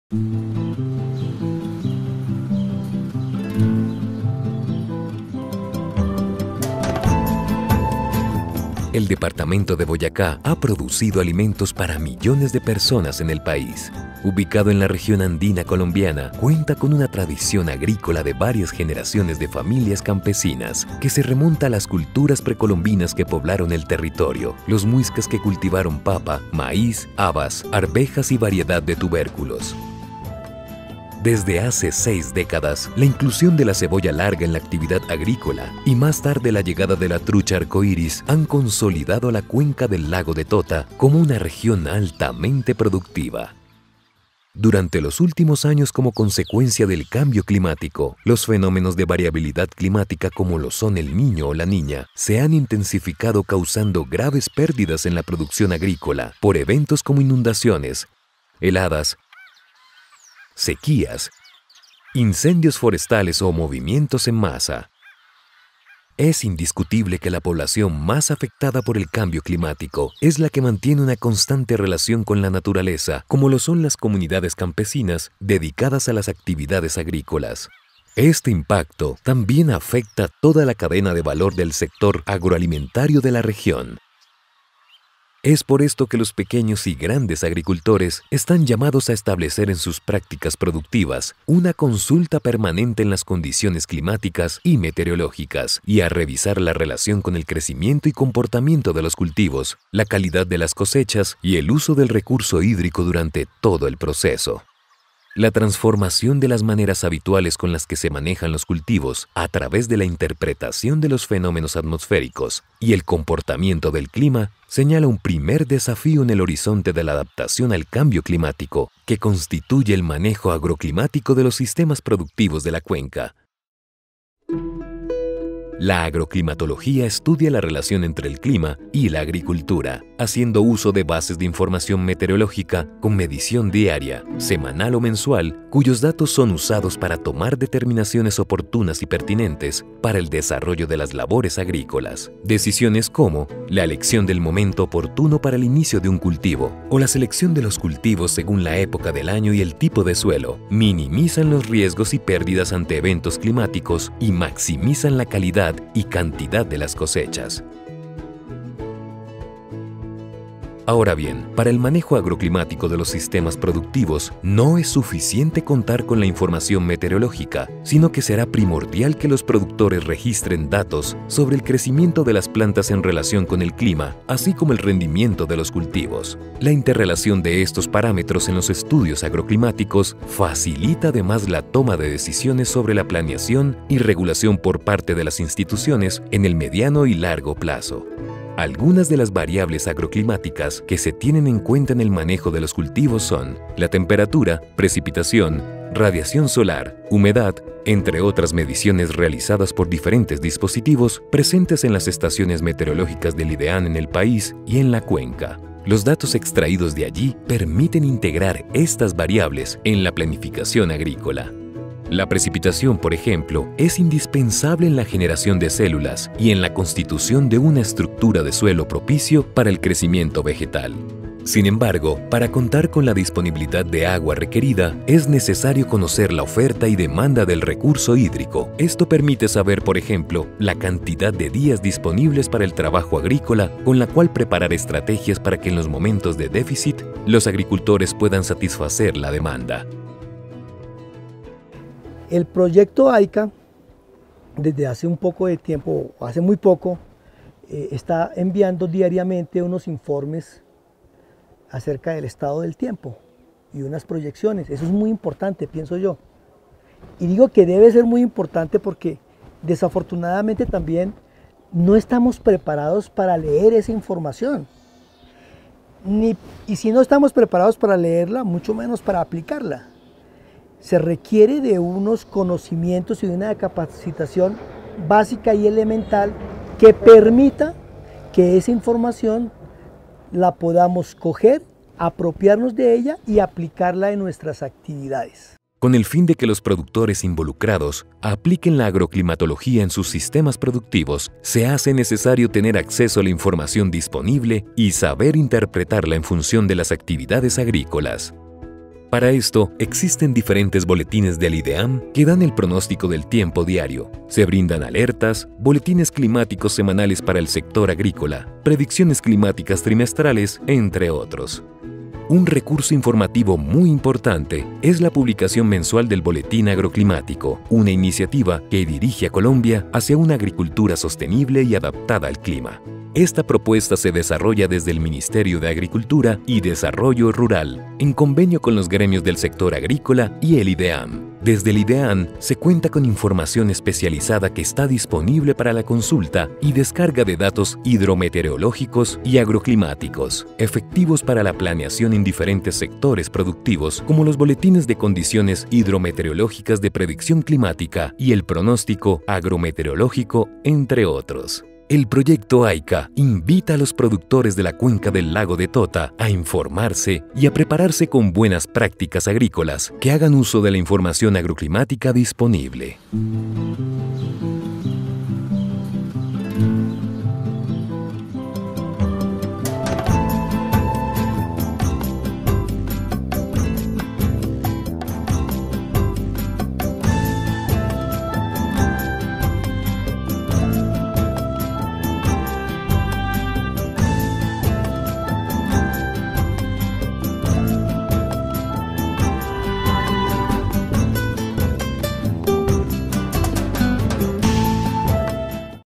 Al día con el clima - AICCA Colombia
Este material busca dar bases conceptuales para la aplicación de la información climática, para el manejo de los recursos y la producción agrícola.